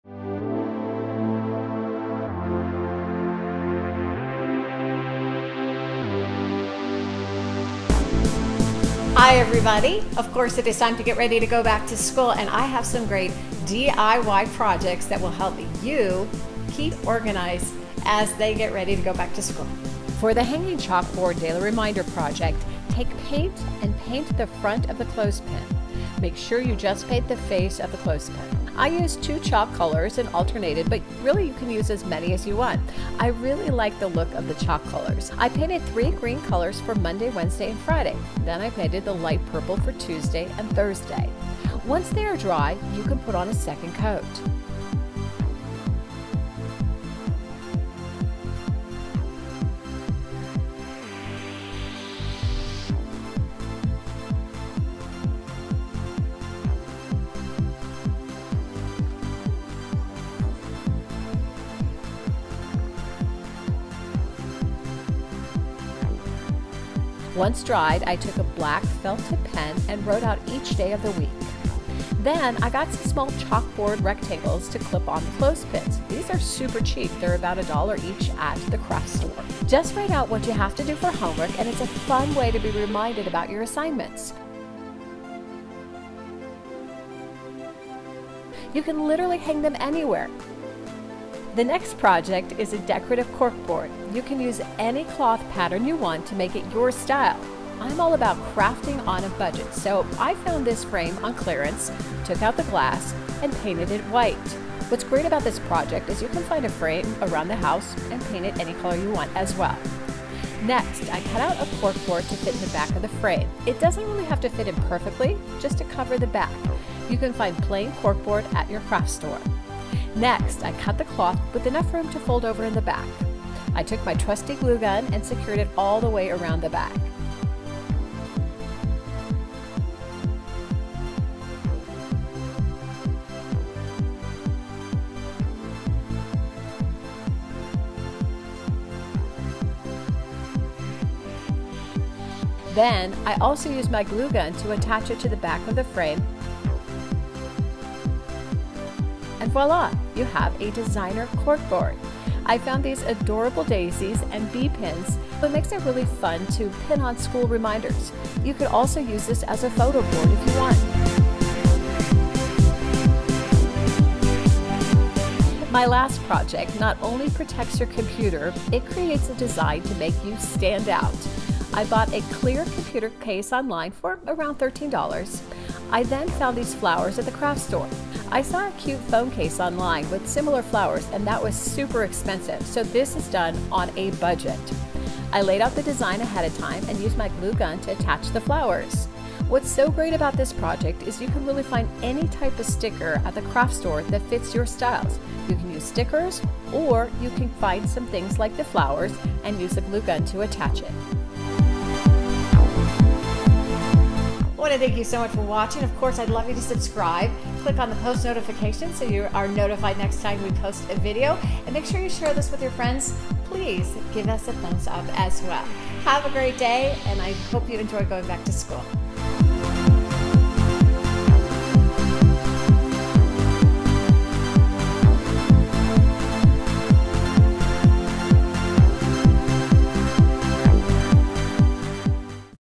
Back to School DIY Hacks and Crafts - Corkboard, Laptop Cover, To-Do Clips
It is time to get ready to go back to school and there is nothing more important than getting organized. Here are several ideas to go back to school in style and stay organized at the same time.